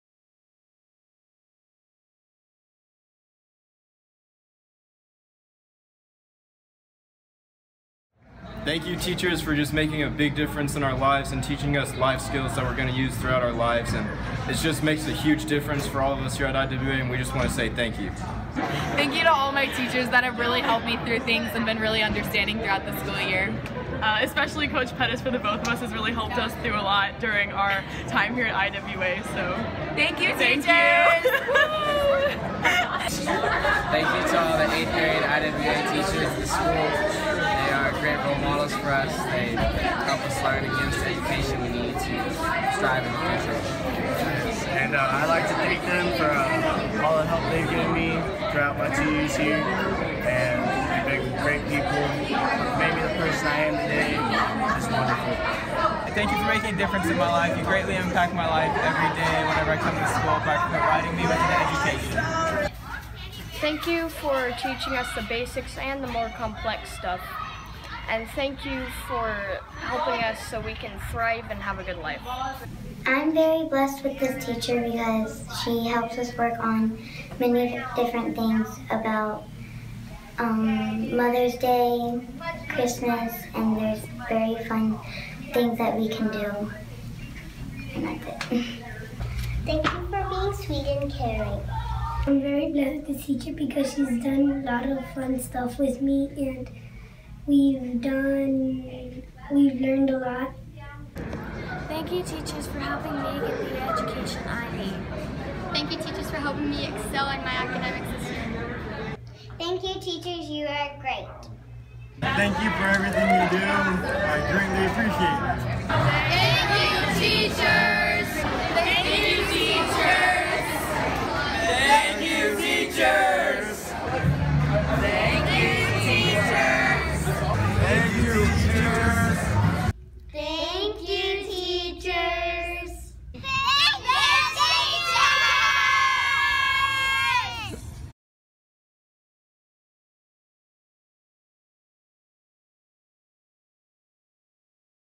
IWA Students Say Thank You!
DescriptionThe IWA teachers give us so much. A boost of confidence when we really need one. Extra help when we’re having trouble. A welcoming presence when everything else seems out of control. And though we know we can’t ever thank them enough, we can take a moment to say Thank You!